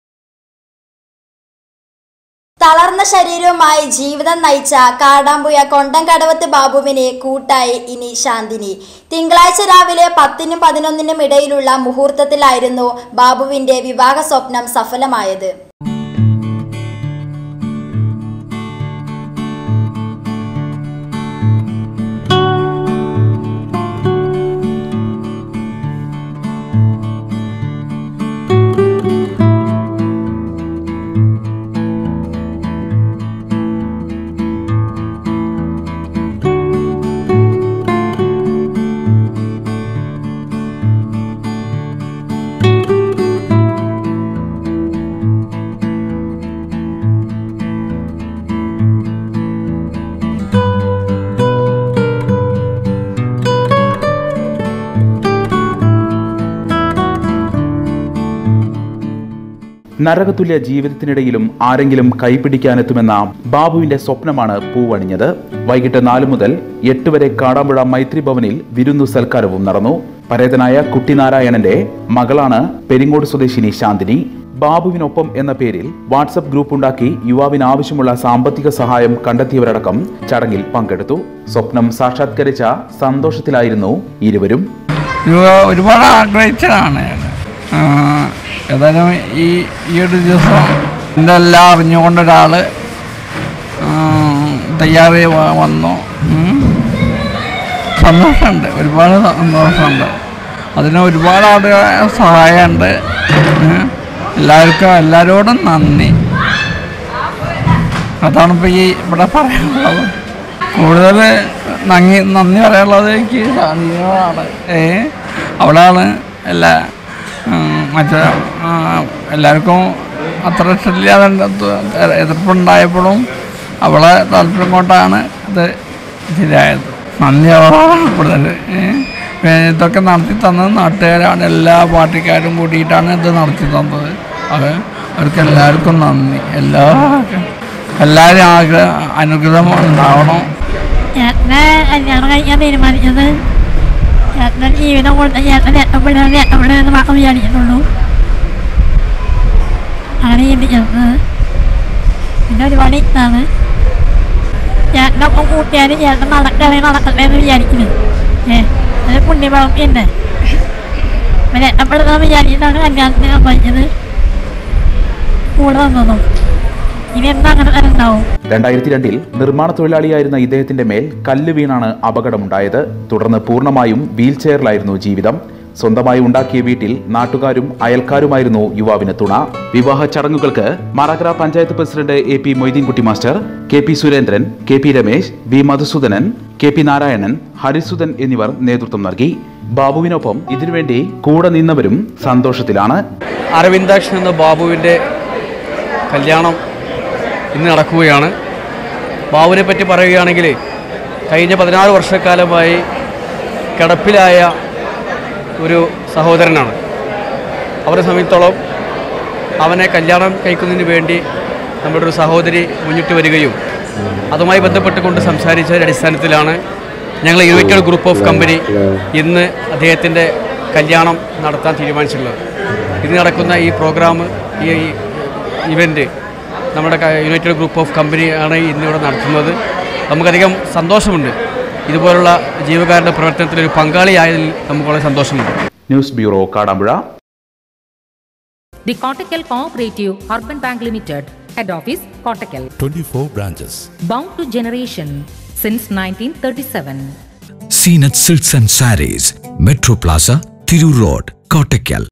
ബാബുവിന് ഇത് സ്വപ്ന സാഫല്യം...ജീവിതത്തിൽ ഇനി ബാബുവിന് ശാന്തിനിയുടെ കൂട്ട് - Kerala Special Wedding
ബാബുവിന് ഇത്  സ്വപ്ന സാഫല്യം...ജീവിതത്തിൽ ഇനി ബാബുവിന് ശാന്തിനിയുടെ കൂട്ട്...